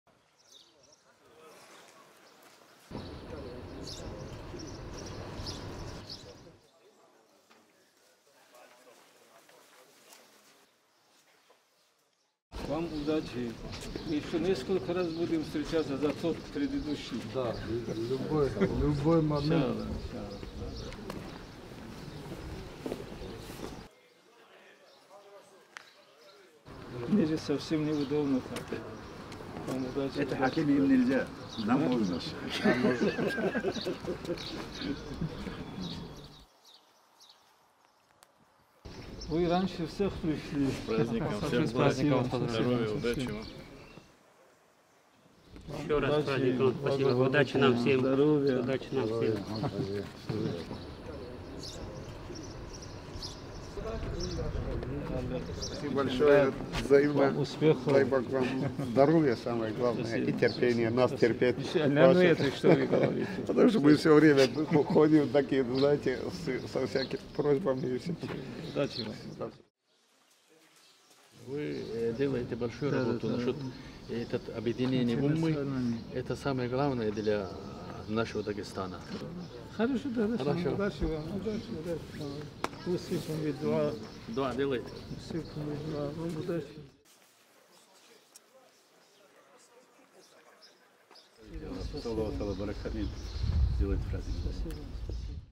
Салман Дадаев поздравил муфтия Дагестана с Ураза-байрам
Сегодня Муфтия РД шейха Ахмада Афанди посетили представители органов власти, религиозные и общественные деятели.

В числе гостей был мэр Махачкалы Салман Дадаев, представители министерств и ведомств республики.

Они поздравили духовного наставника со светлым праздником Ураза-байрам.

Стоит отметить, что во дворе дома Муфтия РД был накрыт праздничный стол, за который мог сесть любой желающий и присоединиться к трапезе.